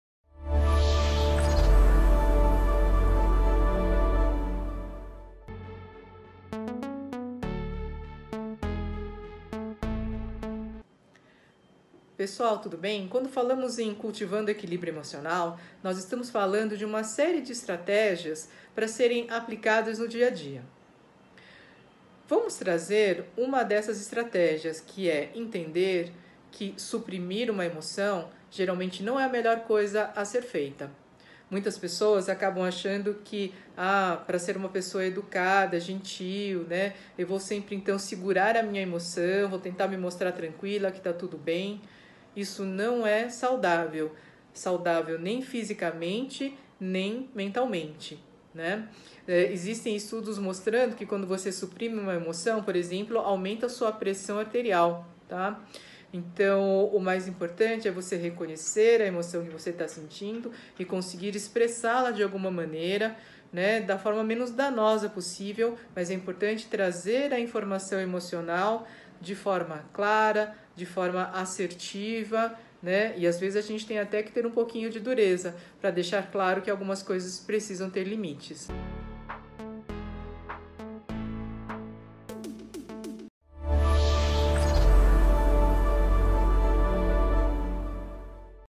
Elisa Kozasa - Por que e como cultivar o equilíbrio emocional?
Elisa Kozasa é Neurocientista,  Fellow do Mind and Life Institute e professora da Palas Athena. Foi treinada por Paul Ekman, Eve Ekman e Alan Wallace para ministrar o treinamento Cultivating Emotional Balance.

Em agosto ela ministrará na Palas Athena o curso on-line  "Cultivando o Equilíbrio Emocional".